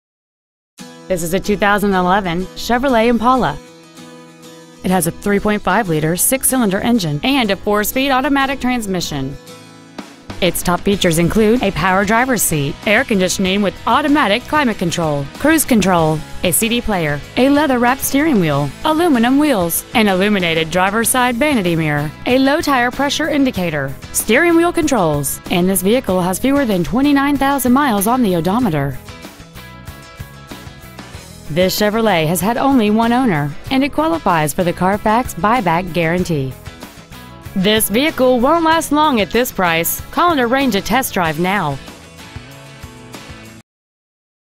2011 Chevrolet Impala Duluth GA 30096
Year : 2011
 Make : Chevrolet
 Model : Impala
 Engine : Gas/Ethanol V6 3.5L/214
 Trans . : Automatic
 Exterior : Summit White
 Miles : 28,601
 Interior : Ebony
 RICK HENDRICK CHEVROLET
 3277 Satellite Blvd
 Duluth , GA 30096
 Preowned 2011 Chevrolet Impala Duluth GA